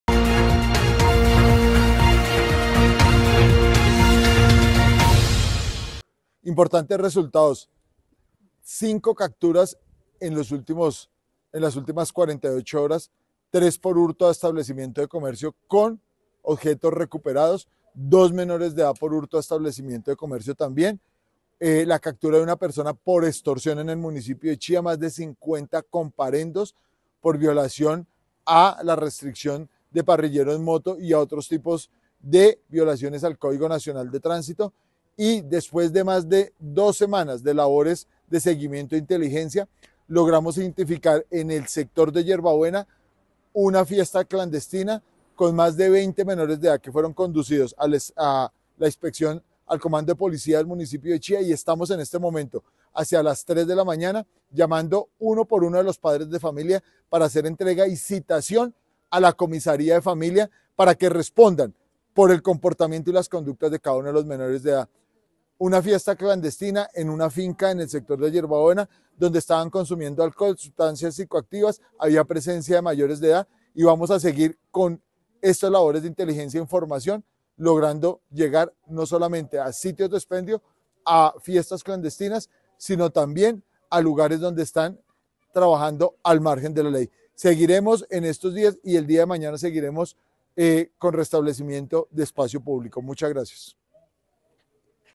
Balance de seguridad por parte de la administración municipal de Chía
Cinco capturados, entre ellos uno por extorsión; más de 50 comparendos por violación a la medida de parrillero en moto, una fiesta clandestina con más de 20 menores de edad y el sello a un establecimiento por exceder los horarios de cierre fueron los resultados que dejaron los operativos de seguridad en Chía este fin de semana.